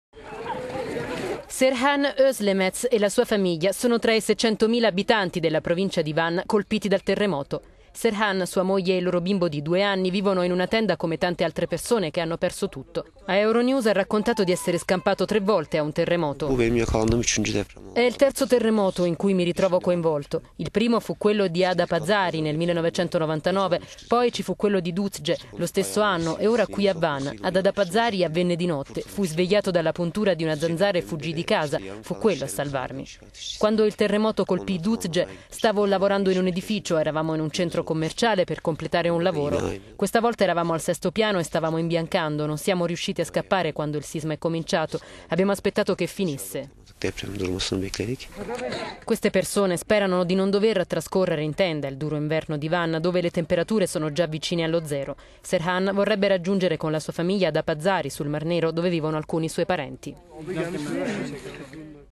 Turchia: tra i superstiti di Van
Serhan Ozlemez e la sua famiglia sono tra i 600mila abitanti della provincia di Van colpiti dal terremoto.

Serhan, sua moglie e il loro bimbo di due anni vivono in una tenda come tante altre persone che hanno perso tutto. A Euronews ha raccontato di essere scampato tre volte a un terremoto.